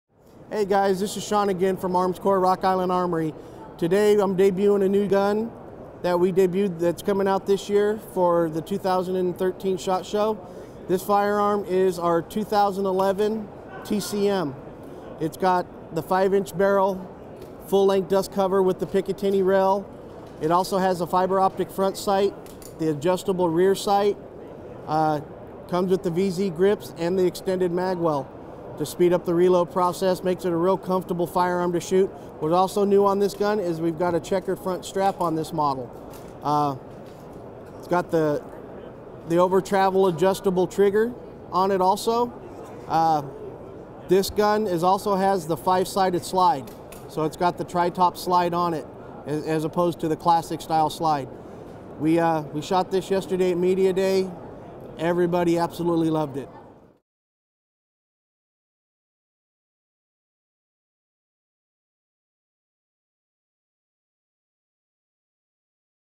SHOT Show 2013: Rock Island Armory - TCM 22 FS 2011 G10 Grip
Master gunsmith Shawn Fairburn from Rock Island Armory takes a second to overview the flagship product for 2013: the TCM 22 FS 2011 Tactical G10 Grip.